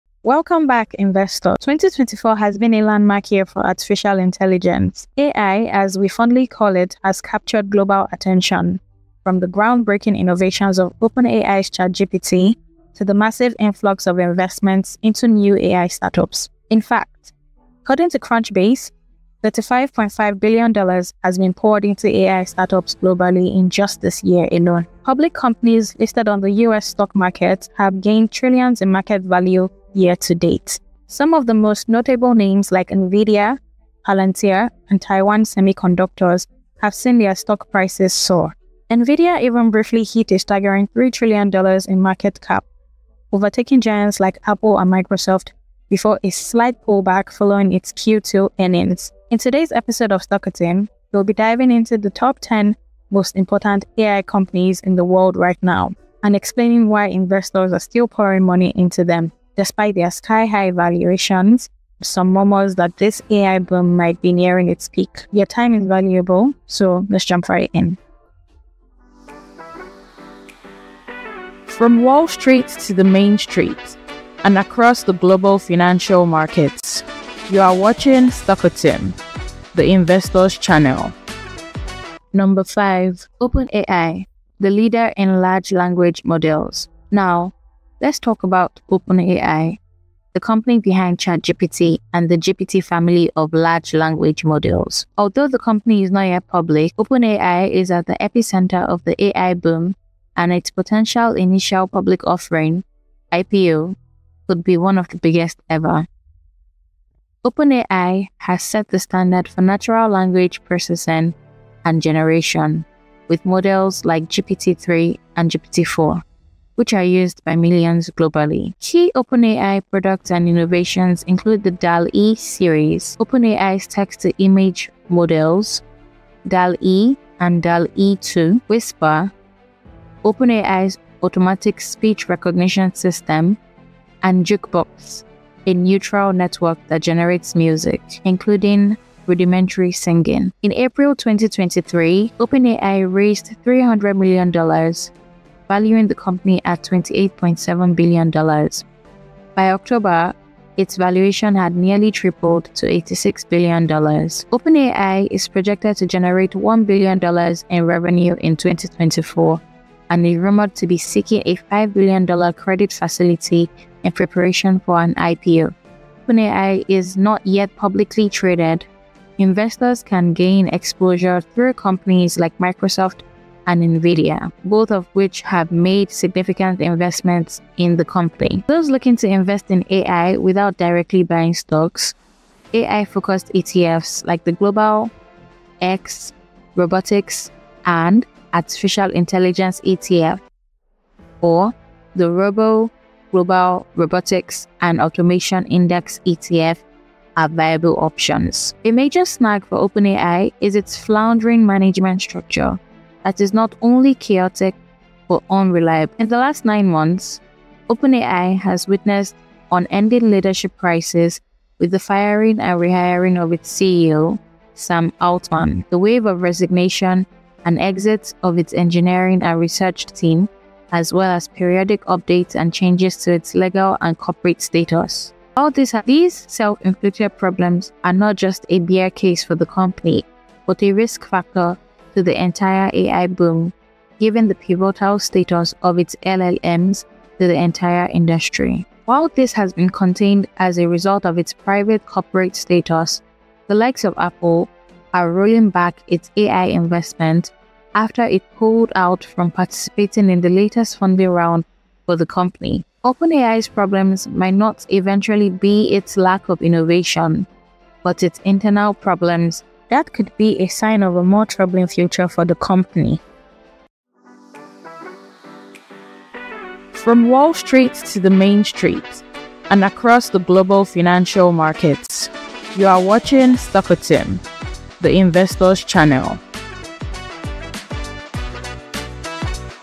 Is OpenAI Stock worth Buying when it Goes Public?
Explore OpenAI’s game-changing innovations in AI, from GPT language models and DALL-E’s image generation to Whisper’s speech recognition and Jukebox’s music creation! With a valuation jump from $28.7 billion to $86 billion, OpenAI is making waves, even without being publicly traded. We’ll discuss ways to gain exposure to OpenAI’s growth through investments in Microsoft, NVIDIA, and top AI ETFs.

Happy investing!